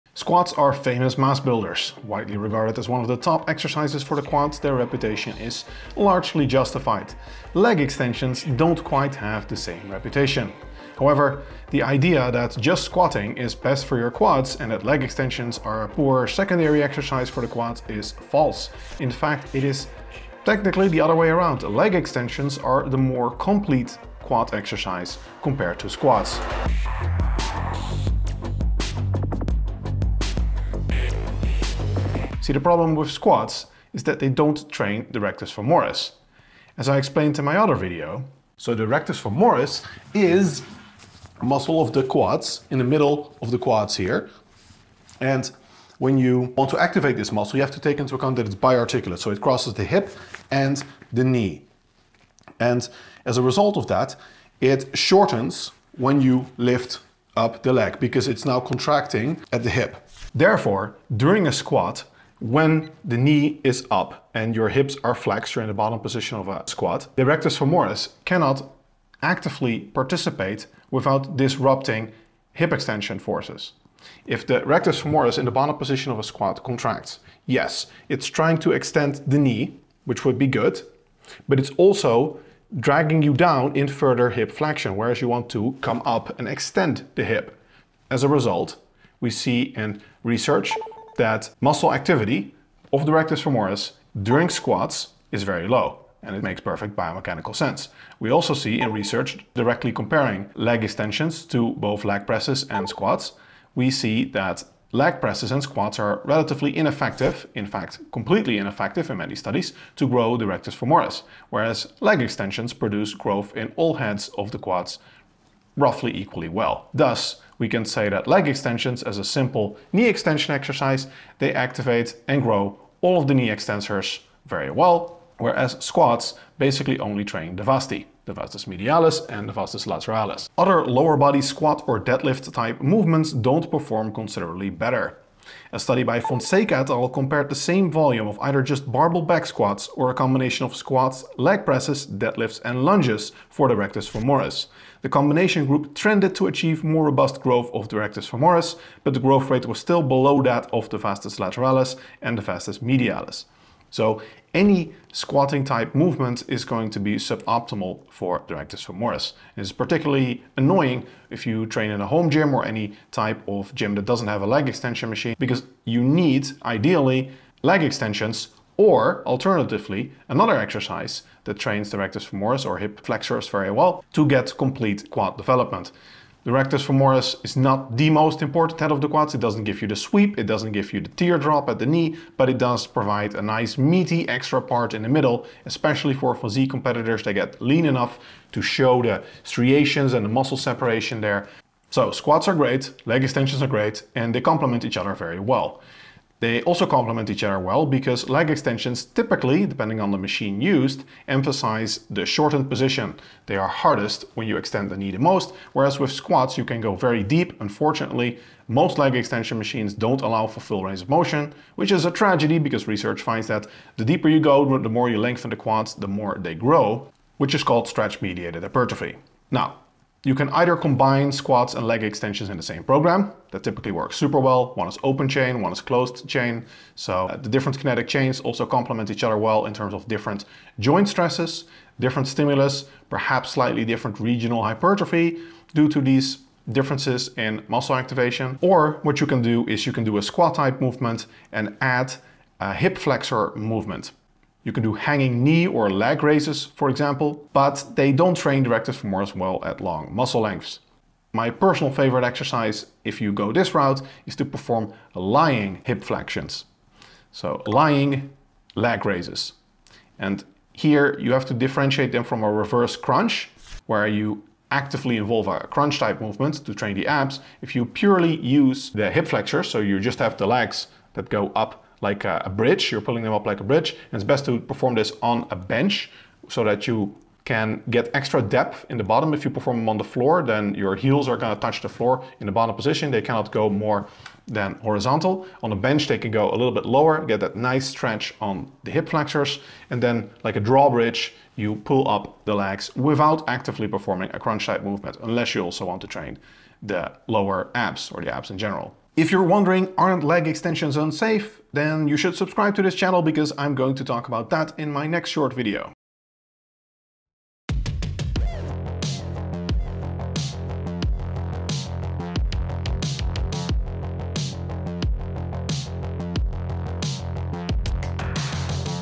Why leg extensions beat squats for the quads [study]
For more free fitness tips, check out my free email course

Chapters:
00:00 Introduction
00:32 The problem with squats
01:29 Studies
02:33 Optimal way to train the quads